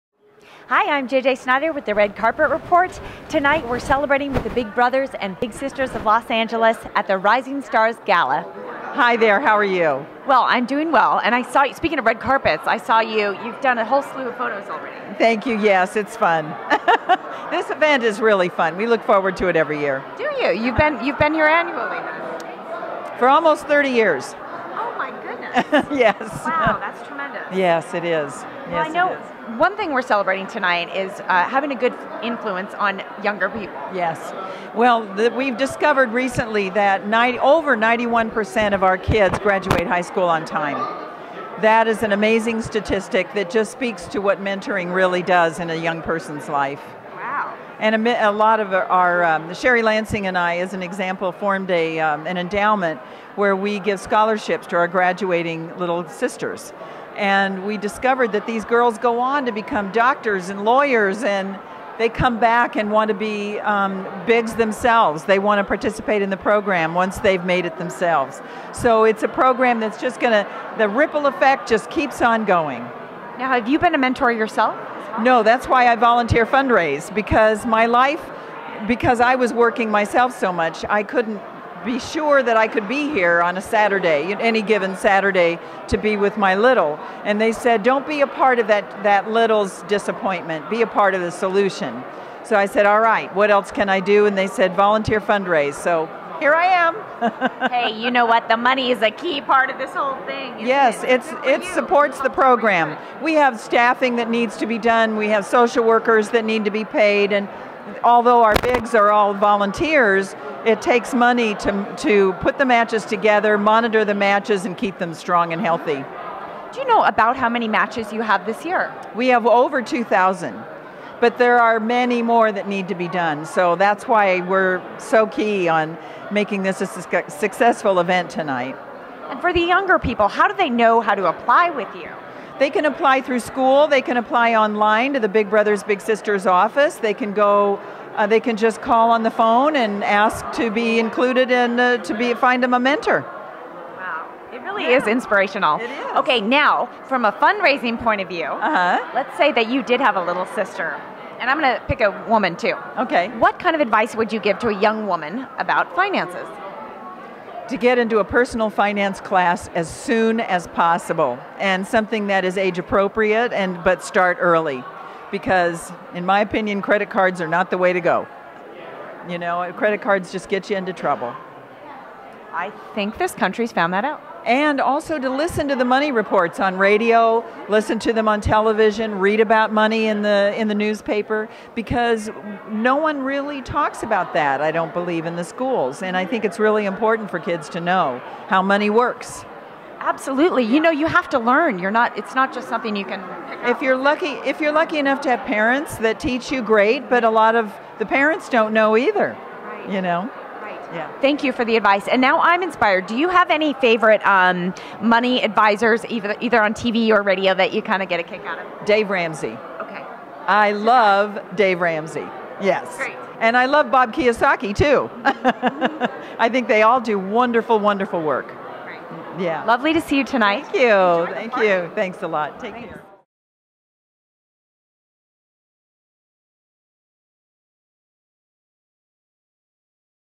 Sarah Purcell at Big Brothers Big Sisters 2012 Rising Stars Gala
Mingle Media TV and Red Carpet Report host, JJ Snyder, were invited to come out and cover the 2012 Rising Stars Gala at the Beverly Hilton Hotel in Beverly Hills in support of Big Brothers Big Sisters of Greater Los Angeles (BBBSLA).

At the event BBBSLA honored outstanding members of the Los Angeles community who inspire and portray positive role models for young people.  Among those recognized were Sumner M. Redstone, Executive Chairman of Viacom and CBS Corporation, who received the Walt Disney Man of the Year Award; Dana Walden, Chairman of Twentieth Century Fox Television, received the Sherry Lansing Award; Roshon Fegan, actor, musical artist and producer, received the Rising Star Award; and long-time BBBSLA Guild member Kim Vamos was honored with the Trailblazer Award. Find out more about how you can positively impact a child's life by visiting

There were special performances by Emmy and Golden Globe nominated singer/actor Lea Michele, Amber Riley and Darren Criss, from the award-winning television show GLEE.

Thanks to Katy Sweet & Associates Public Relations for the invitation.Distributed by OneLoad.com